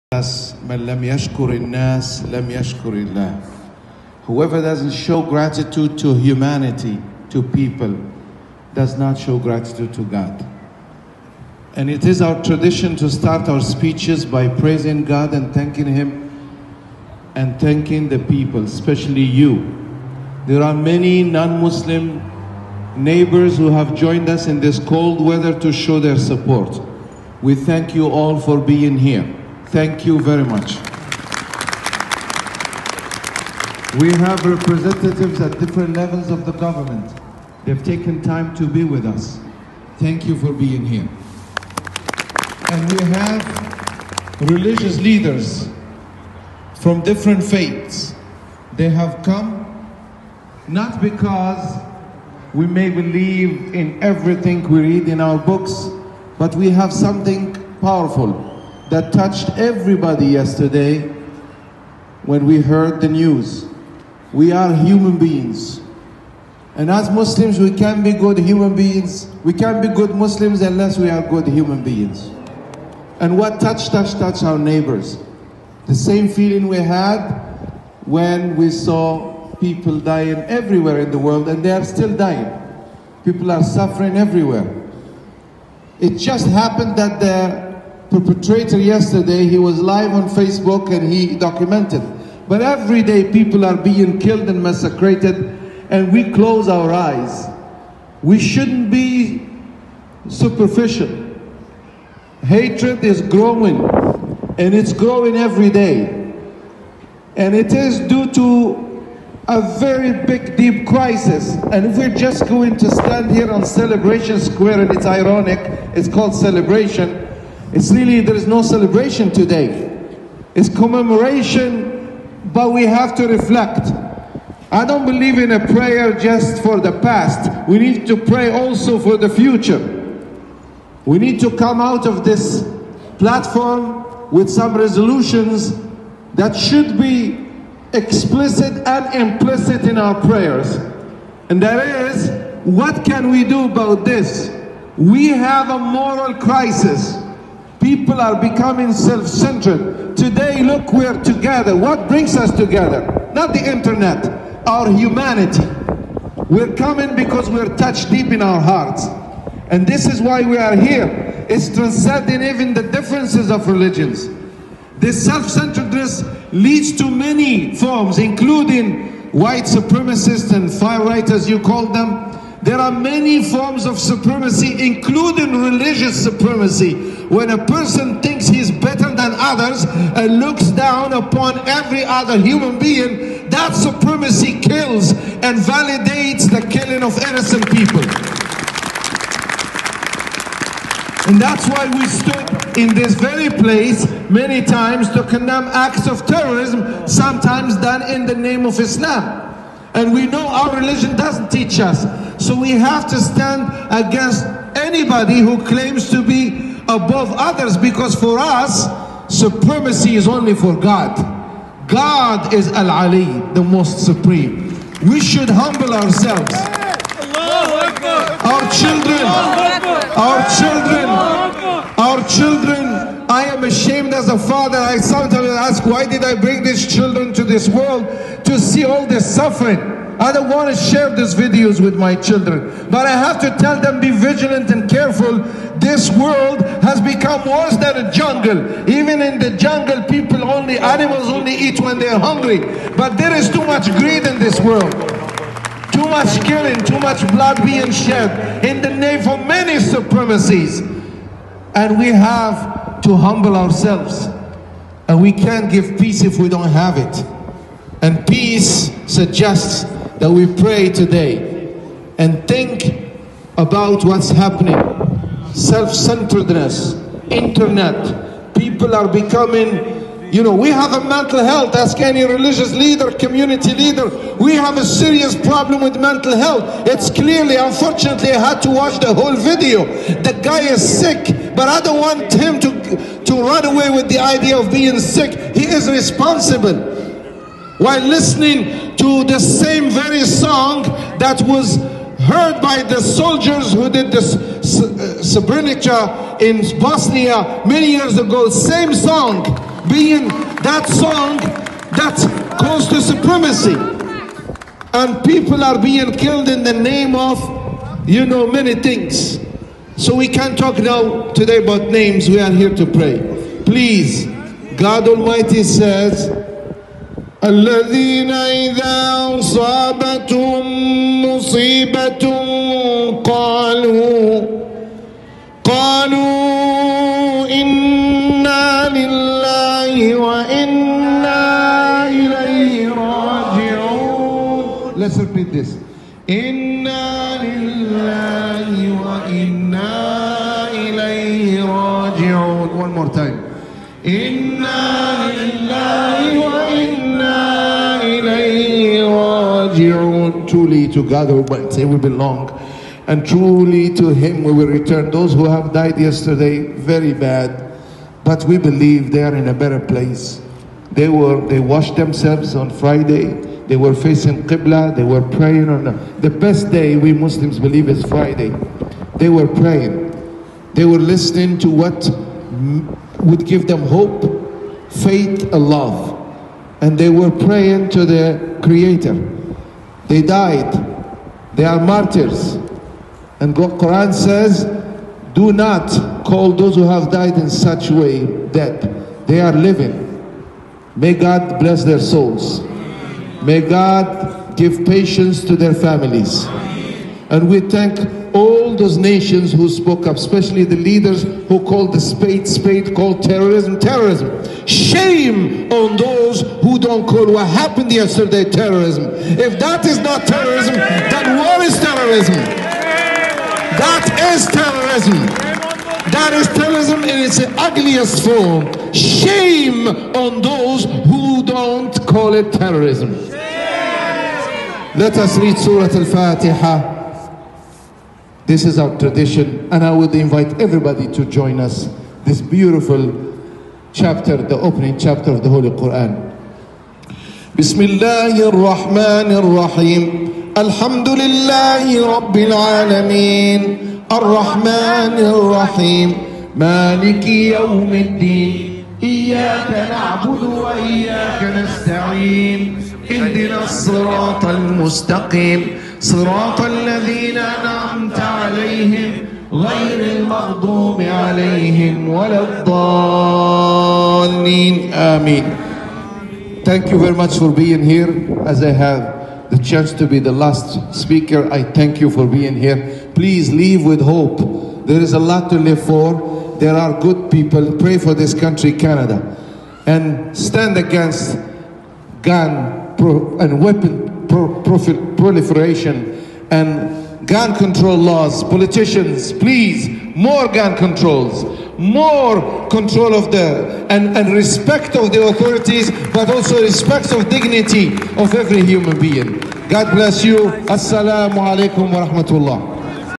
New Zealand Mosque Shooting
Vigil at Square One Mississauga
People gathered outside of Mississauga city hall on Saturday afternoon to show sympathy for the victims of the New Zealand mosque attacks. Speech by Imam Dr. Hamid Slimi